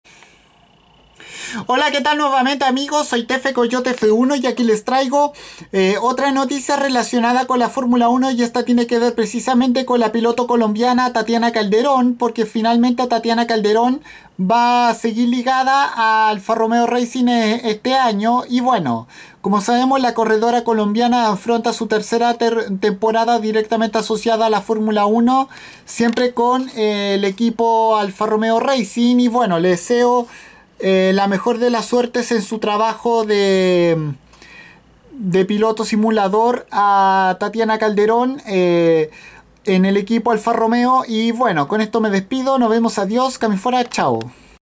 Tatiana Calderón continuará con Alfa Romeo Racing en 2019
La piloto colombiana seguirá con el equipo con sede en Hinwill